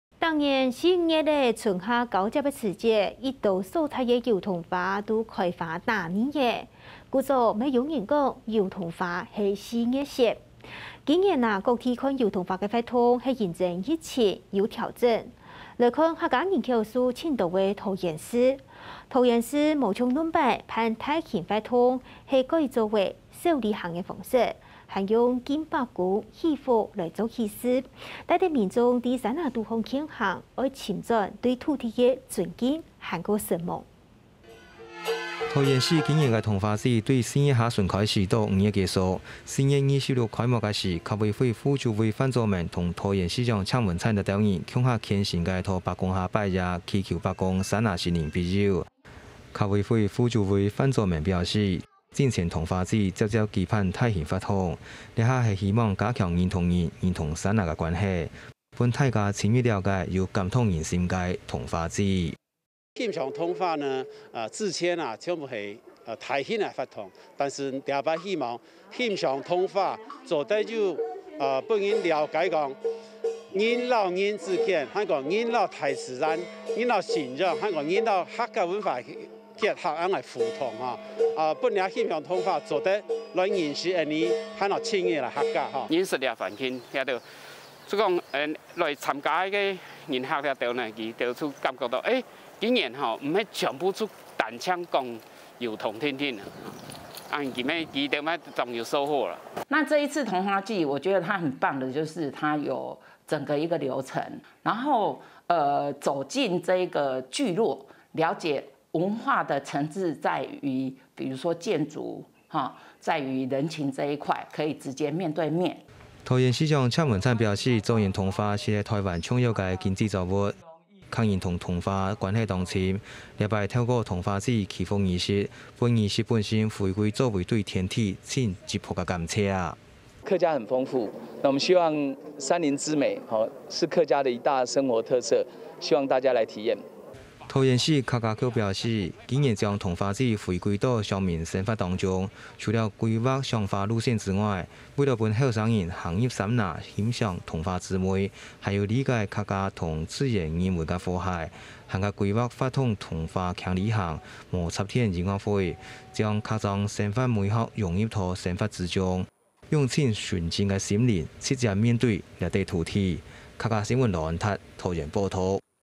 改為輕旅行型態 桐花祭回歸對土地的感謝【客家新聞20220531】
每年4、5月春夏交替時節，各地客庄附近的桐花紛紛盛開，桐花也因此擁有了四月雪的稱號，而今年各地欣賞桐花，卻因疫情關係有所調整，以客家人口數最多的桃園市為例，桃園市將以往舉辦大型活動方式，改成為輕旅行型態，並以拜伯公祈福祭儀開始，帶領民眾在山林裡健走，回歸對土地最原始的尊敬與感謝。